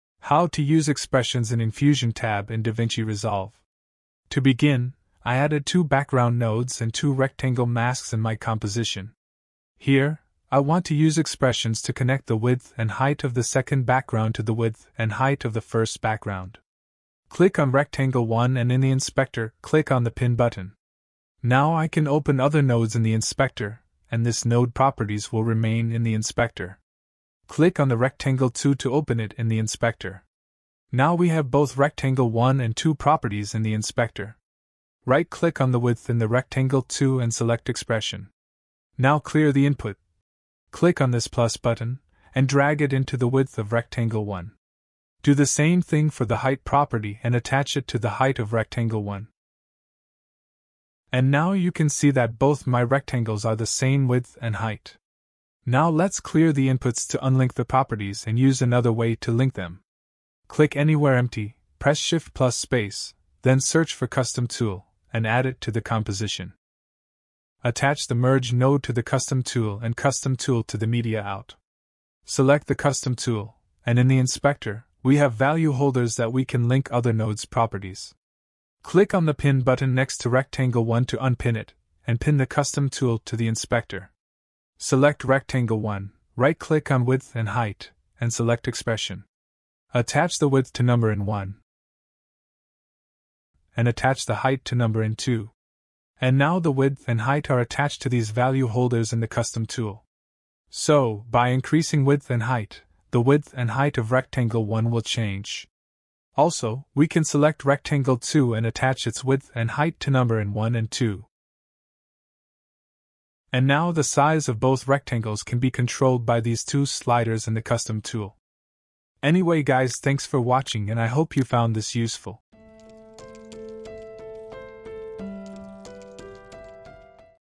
How to Use EXPRESSIONS in Fusion Tab in Davinci Resolve
In Davinci Resolve's Fusion tab, you can use expressions to connect and control node properties. Simply link node properties using expressions or utilize value holders in a custom tool for more flexible control over multiple nodes simultaneously.

How to Use EXPRESSIONS in Fusion Tab in Davinci Resolve Chapters:
00:00 Intro
00:18 pin node to inspector in davinci resolve
00:32 use expressions for property
00:59 use custom tool to link properties